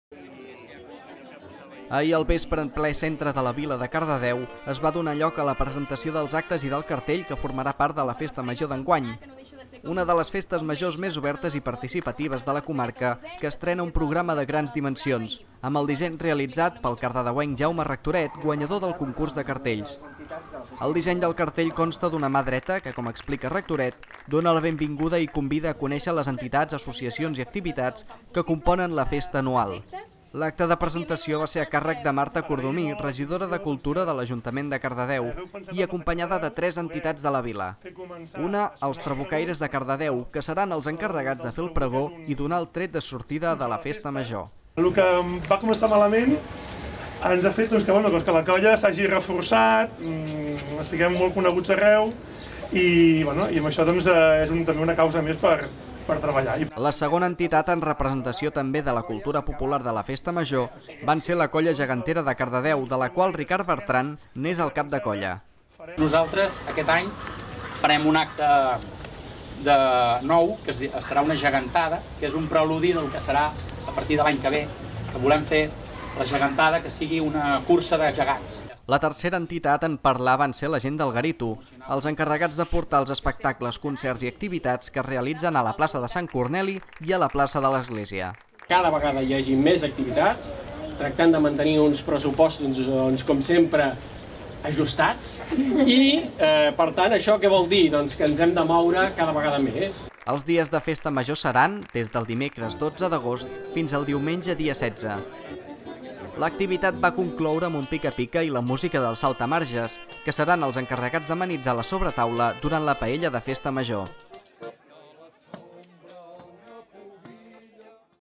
Presentació del cartell i programa de la Festa Major de Cardedeu 2015
Ahir al vespre en ple centre de la vila de Cardedeu es va donar lloc a la presentació dels actes i del cartell que formarà part de la Festa Major d’enguany. Una de les festes majors més obertes i participatives de la comarca.